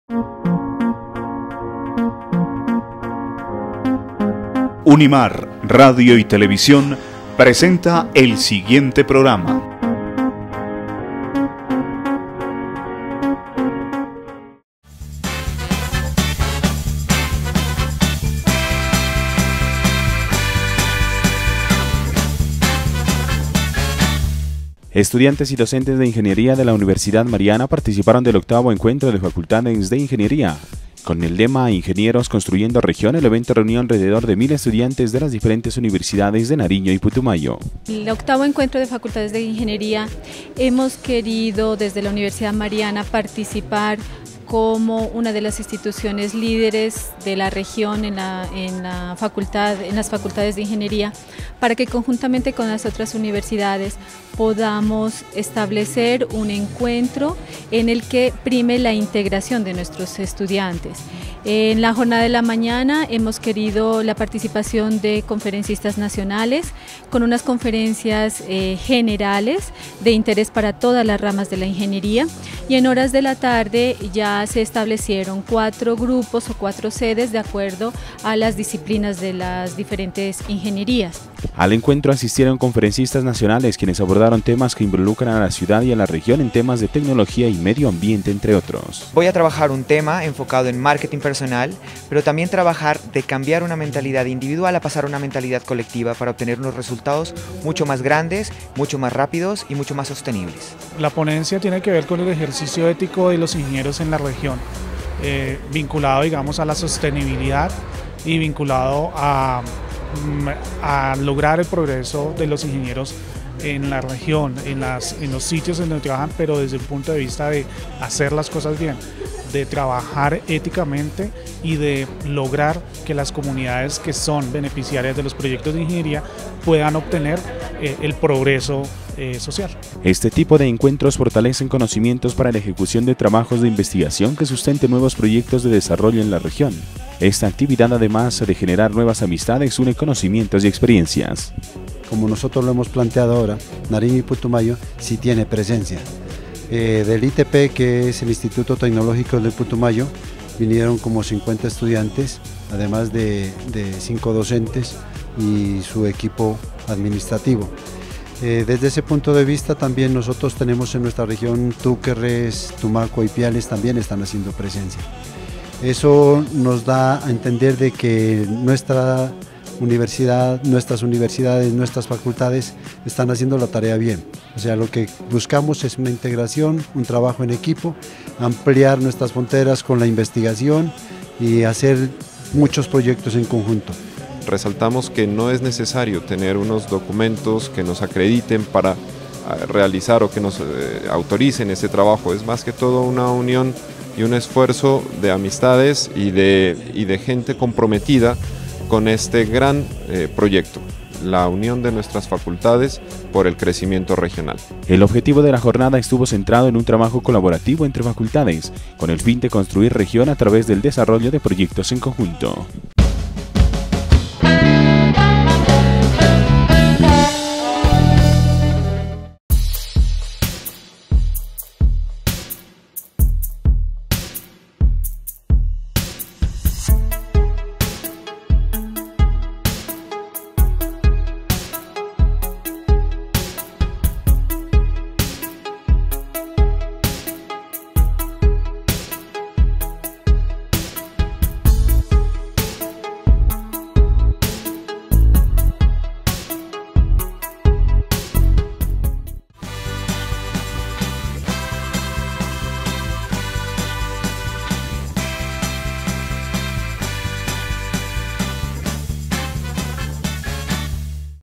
Agenda Unimar 19 de Septiembre 2016
Estudiantes y docentes de Ingeniería de la Universidad Mariana participaron del 8º Encuentro de Facultades de Ingeniería realizado en el club del Comercio de Pasto. Con el lema “Ingenieros Construyendo Región” el evento reunió alrededor de mil estudiantes de las diferentes Universidades de esta ciudad.